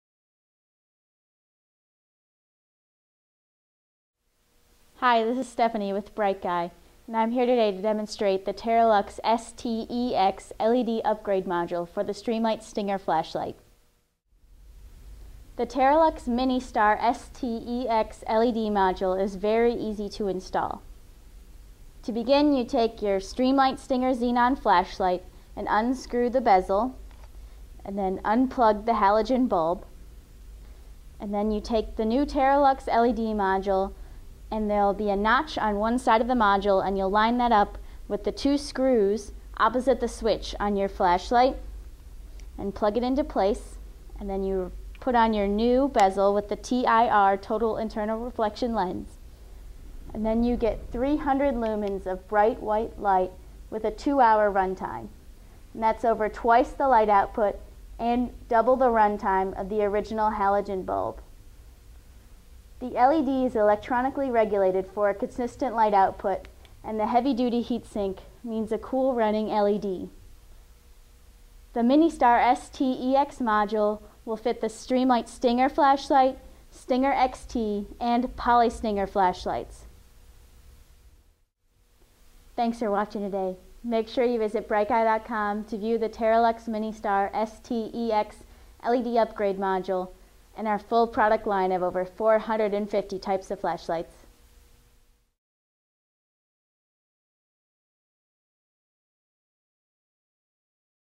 TerraLUX MiniStar ST/EX LED Module for Streamlight Stinger Flashlight
Click the link below to view the TerraLUX ST/EX LED module on BrightGuy.com:

The TerraLUX MiniStar TLE-ST1-EX is a custom engineered LED upgrade kit for the Streamlight Stinger®, Stinger XT® and PolyStinger® flashlights (xenon models only). It converts your Stinger, Stinger XT or PolyStinger xenon flashlight into an LED flashlight with 300 lumens and up to 2 hours of runtime per charge. This MiniStar ST/EX features a patented design that includes a heavy duty heat sink that ensures a cool running LED and high quality TIR replacement lens.